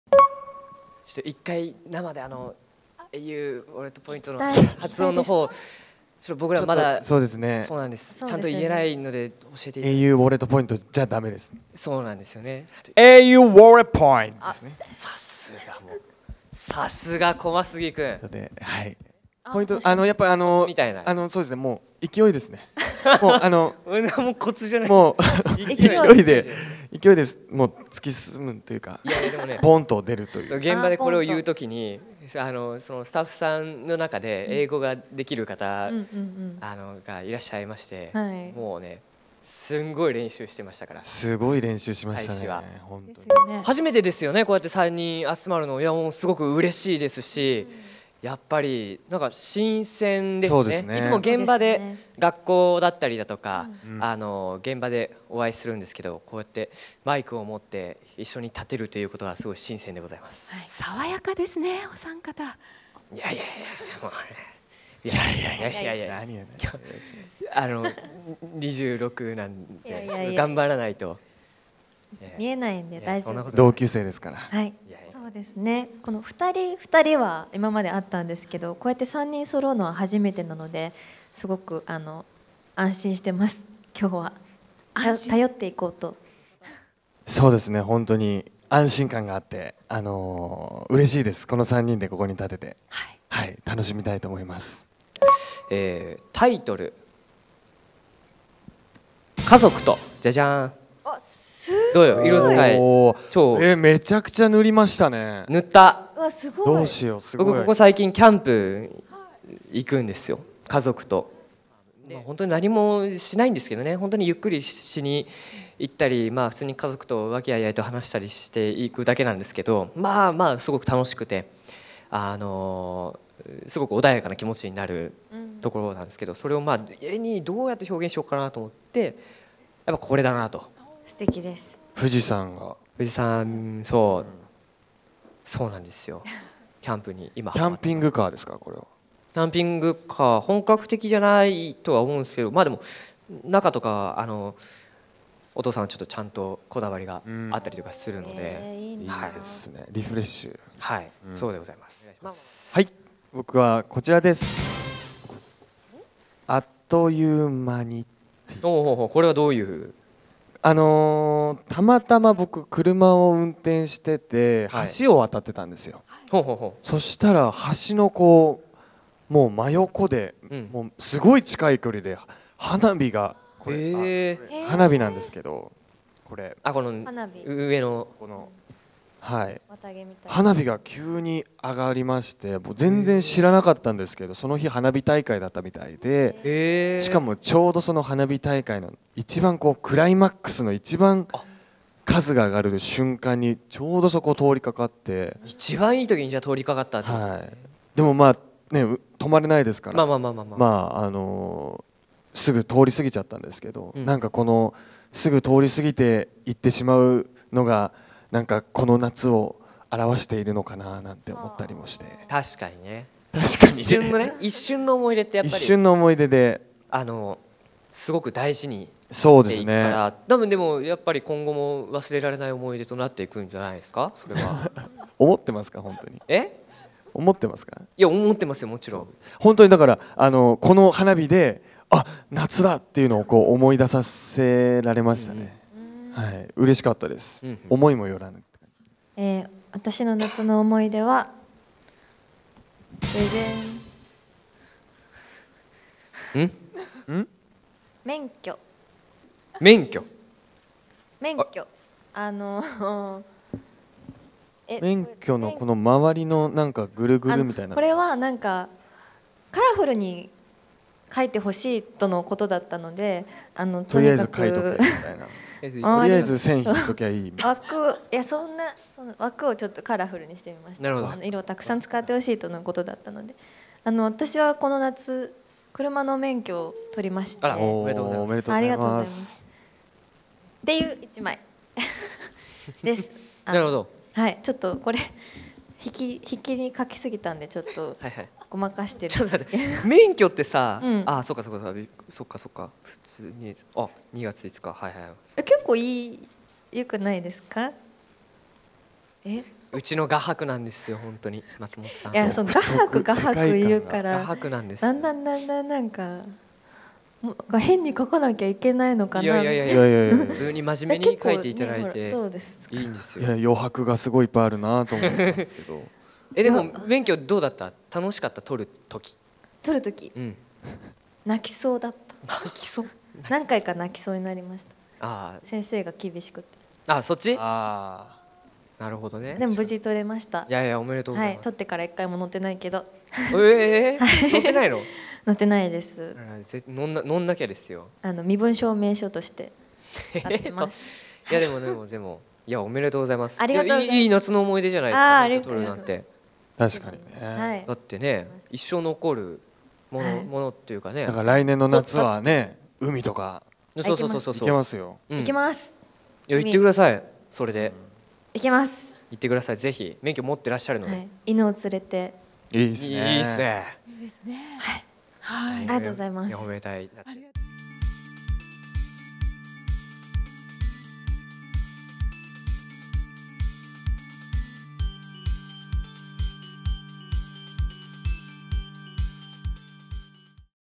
神木隆之介・松本穂香・中川大志 CM「高杉くんシリーズ」の3人がはじめての全員集合であのセリフも生披露！au UNLIMITED WORLD 発表会
｢意識高すぎ！高杉くんシリーズ｣のCMでおなじみの神木隆之介（高杉くん）、松本穂香（松本さん）、中川大志（細杉くん）が登場し、新CMのお披露目やトークセッションが行われた。
イベント中、神木隆之介が中川大志に、CMのあのセリフを無茶ぶりすると、中川大志は全力でセリフを披露し、会場は盛り上がった。
3人の絶妙にゆるいトークは、CMの空気感のままで、今後の新CMにも期待が高まった。